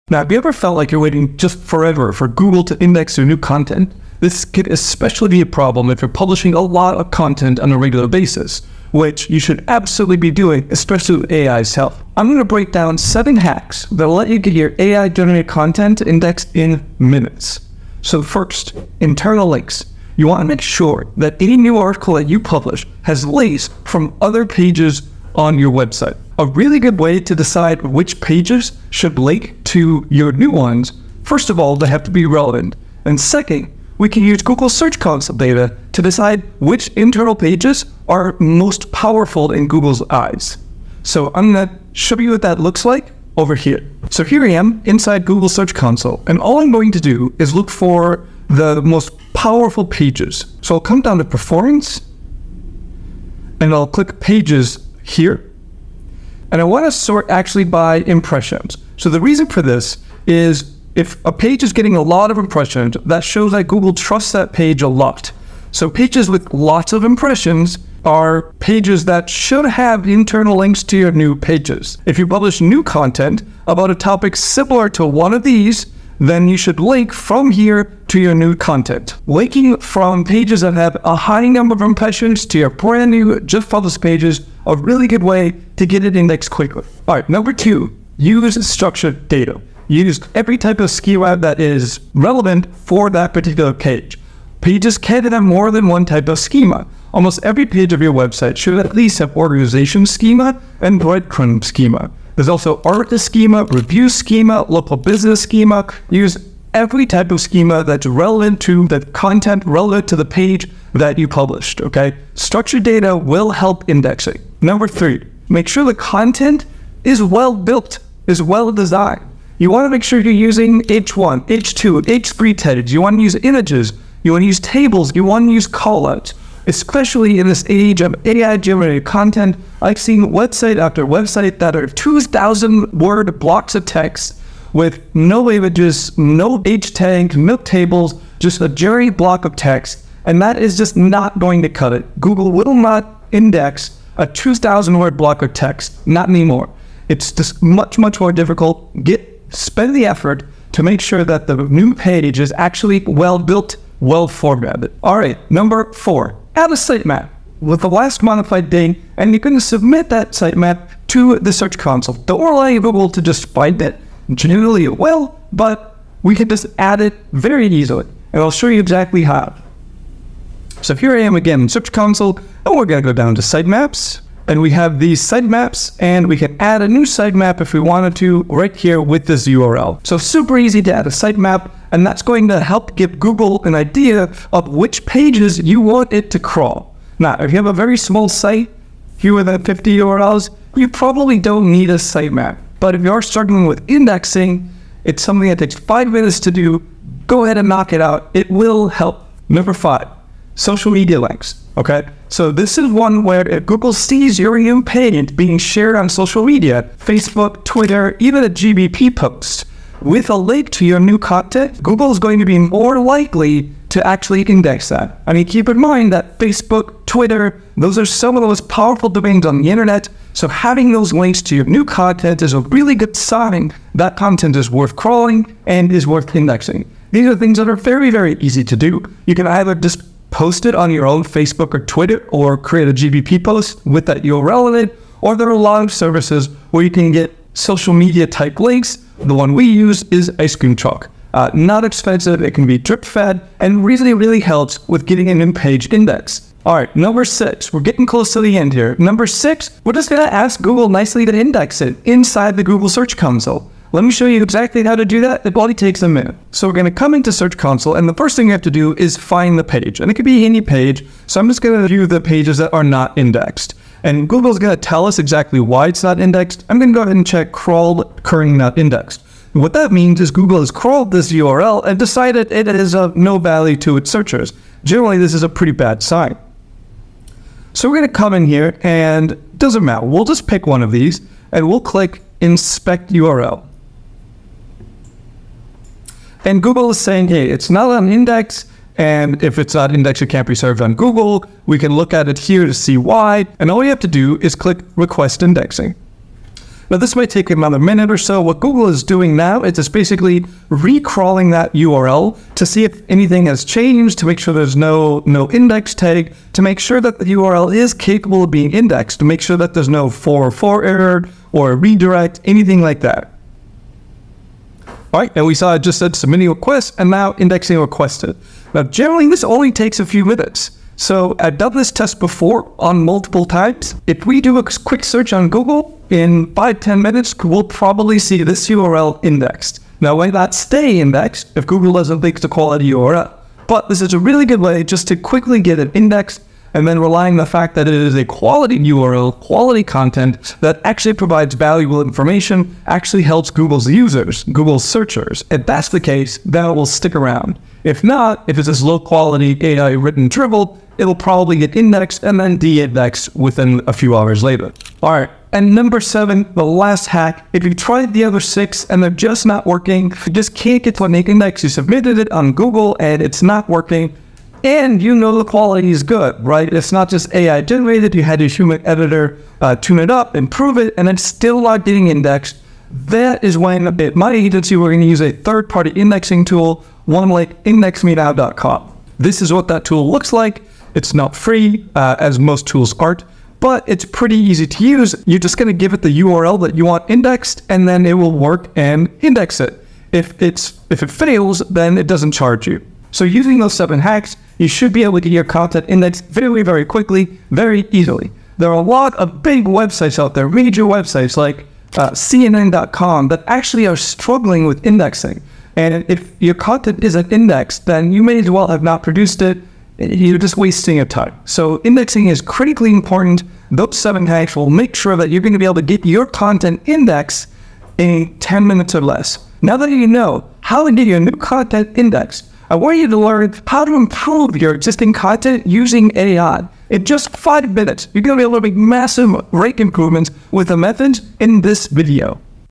Instant SEO Results 👀 Index AI Content in 2 Minutes ⭐ (7 Methods)
_ 🚀 Struggling to get your AI-generated content noticed by Google? In this video, I reveal seven powerful methods to ensure your content gets indexed within minutes, not days! 🌐✨

💬 🚀 Check out what I CANT say on Youtube!

Dive deep as I navigate Google Search Console, showcasing real-time strategies that I use to prioritize and boost indexing. From social media boosts to smart sitemaps, every tip is designed to provide actionable insights that you can implement right away.

Discover insider techniques, from leveraging internal links to tapping into the potential of structured data, and even how to directly request Google for quicker indexing. Whether you're a seasoned SEO expert or just starting, these hacks are designed to supercharge your content's visibility almost instantly.

00:00 - Introduction: Unlock Fast Indexing for Your Content
00:17 - Hack #1: Strategic Linking Within Your Site
01:27 - Hack #2: Maximizing Visibility Through Data Tags
02:05 - Hack #3: Essential Elements for Content Recognition
02:30 - Hack #4: Guiding Google to Your Content
03:10 - Hack #5: Social Signals to Speed Up Indexing
04:00 - Hack #6: Prompting Google for Immediate Attention
05:30 - Hack #7: Alternative Tools for Guaranteed Indexing
06:49 - Wrap-Up: Key Strategies to Keep Your Content Visible
07:00 - Next Steps: Enhancing Content Impact with AI

▶️ Watch now to transform your approach and ensure that your content isn't just created, but seen and valued.